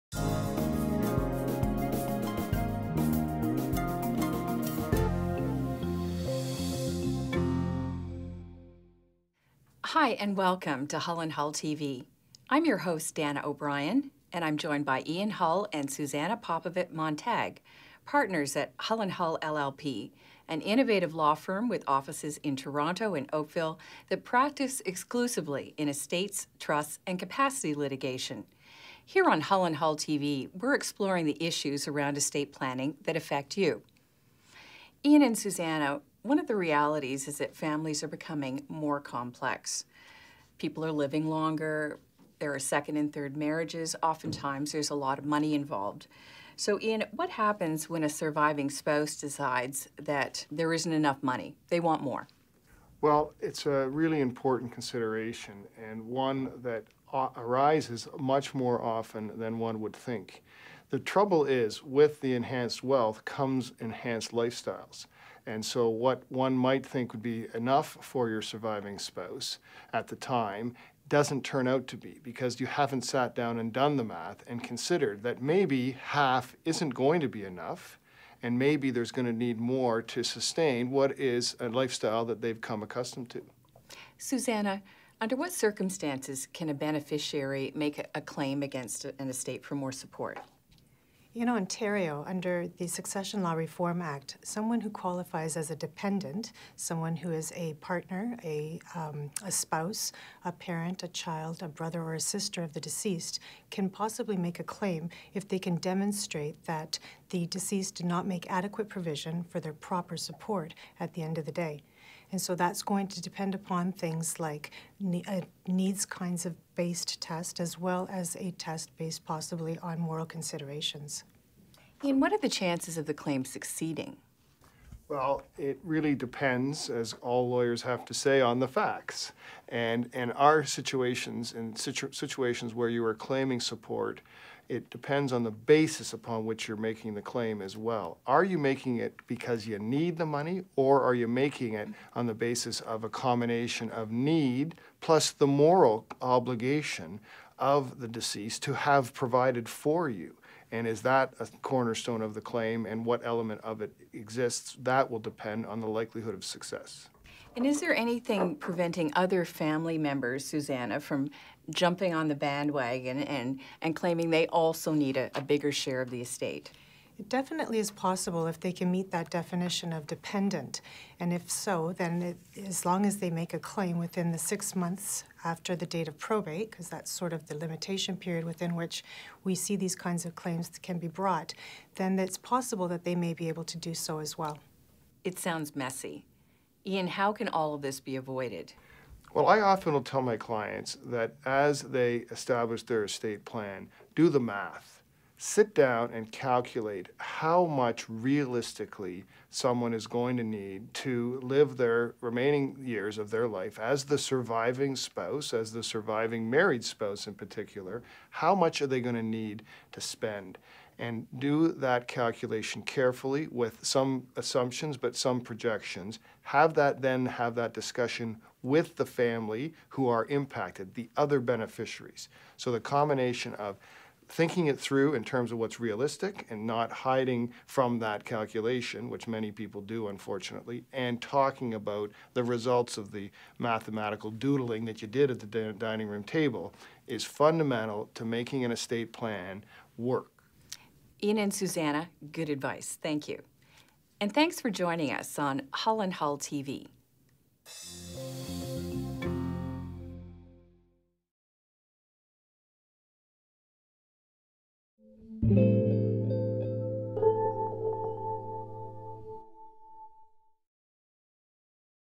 Family Law
Ian Hull & Suzana Popovic-Montag discuss how to handle advanced wealth and the fact that families are becoming more complex.